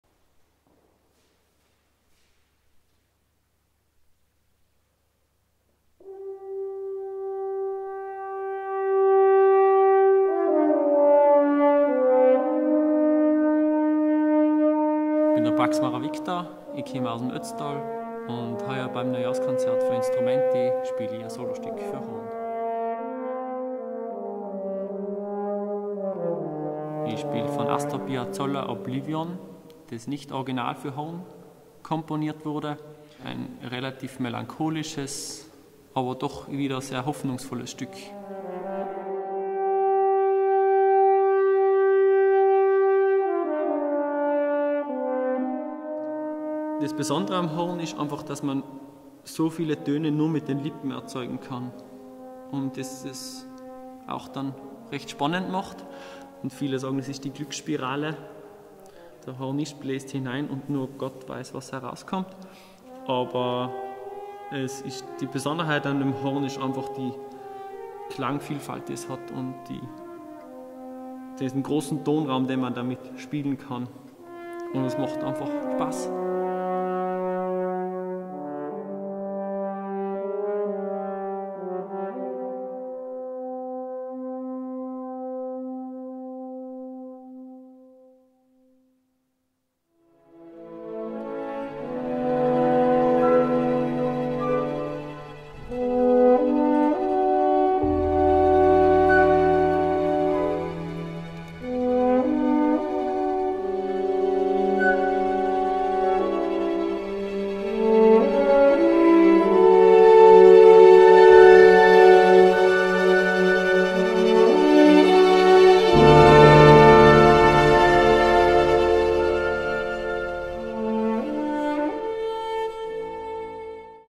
Viktor Praxmarer beim InnStrumenti Neujahrskonzert 2021
Viktor Praxmarer spielt für Sie Horn mit dem Kammerorchester InnStrumenti

am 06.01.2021 - um 10:00 Uhr LIVE
GRATIS ABER NICHT UMSONST
MATTHIAS HOFFMANN singt
VIKTOR PRAXMARER spielt das Horn

THOMAS LACKNER moderiert

CONSTANZE GABRIEL & LUKAS STEINEGGER tanzen
GERHARD SAMMER leitet

Video: Carmen Treichl

VIKTOR InnStrumenti H264 MASTER V02